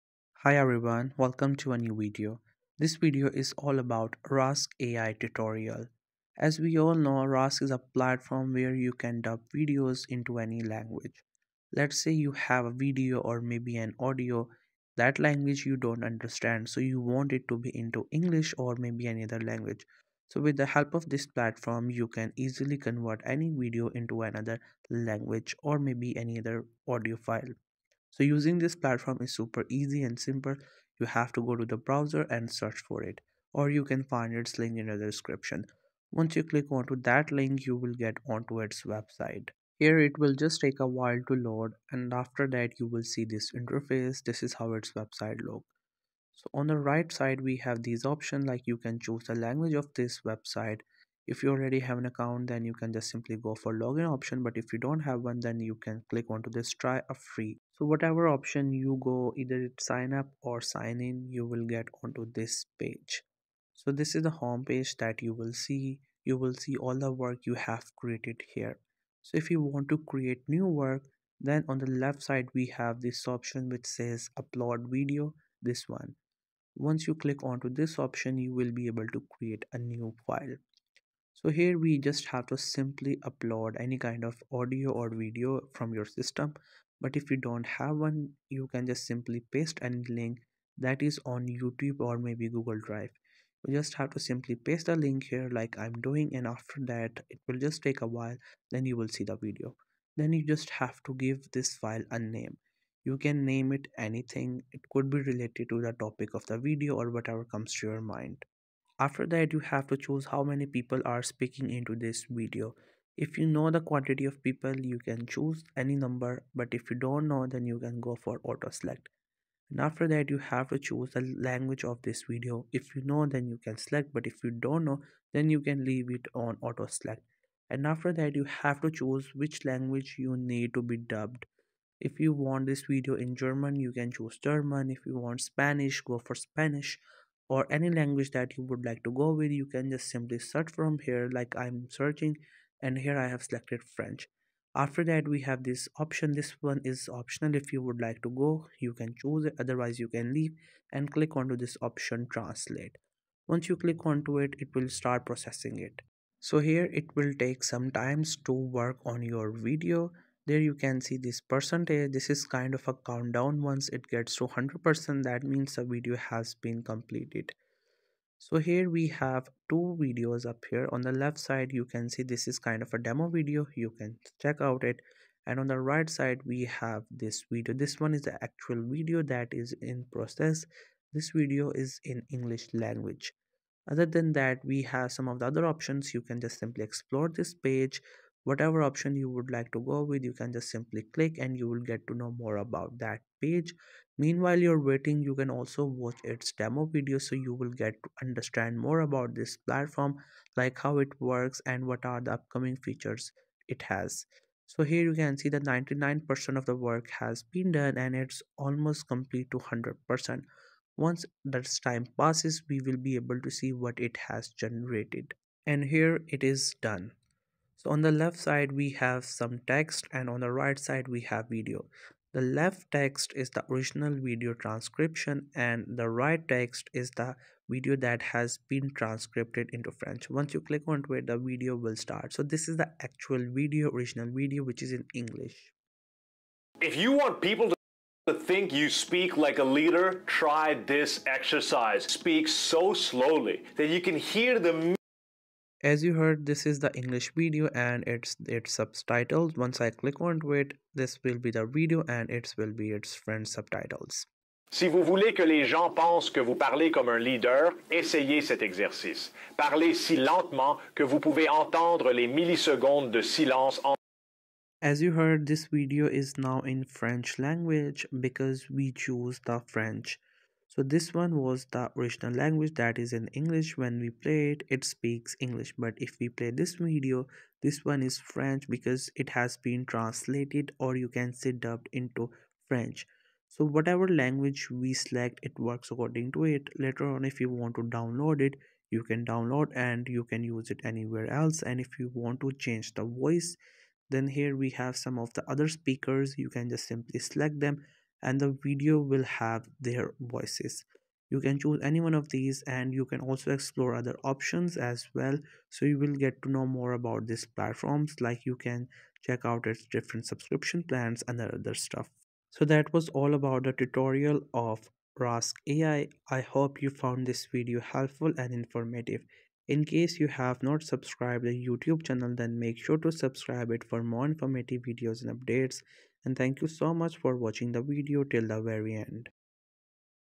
Rask Ai Tutorial (Step by Step)│Ai Hipe
Welcome to Rask Ai Tutorial .
Here is Rask Ai Tutorial . Elevate your productivity game with our in-depth Rask AI tutorial! 🚀 Join us as we unravel the features that make Rask AI a powerhouse for task management and optimization. From smart automation to personalized insights, this visual guide ensures you harness the full potential of Rask AI. Ready to transform your workflow? 🤖💼